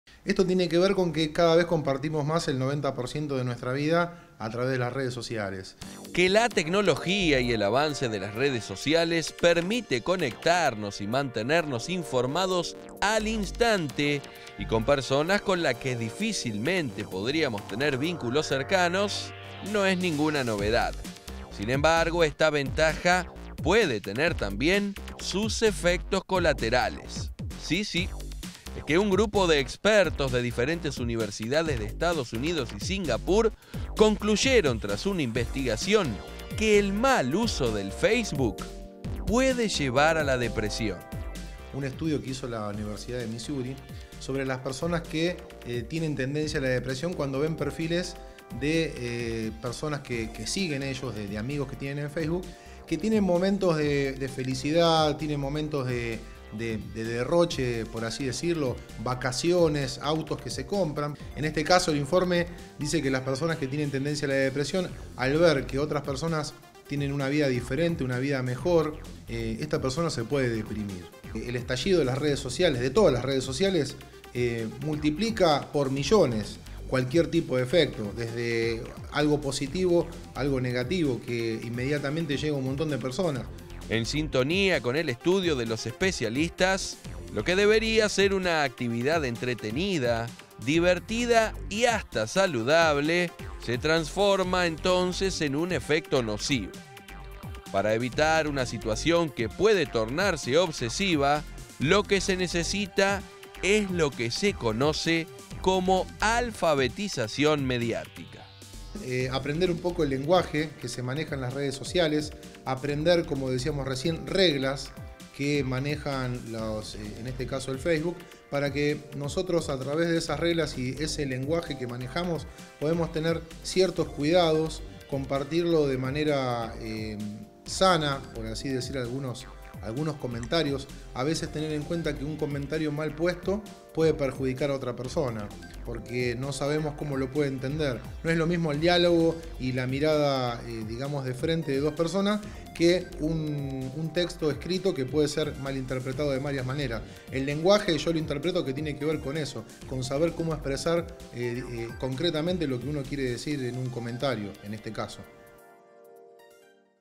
EFECTOS COLATERALES DE LAS REDES SOCIALES
Aseguran que el mal uso de Facebook puede llevar a la
depresión. Así concluye un estudio llevado a cabo por
profesores de las universidades de Missouri, en Estados
Unidos, y Nanyang, en Singapur, en el que se detalla
que cuando los usuarios invierten demasiado tiempo en inspeccionar las publicaciones de carácter positivo de sus amigos en esta red social acaban sintiendo envidia. El especialista en tecnología Alberto Pietrobón analiza este tema.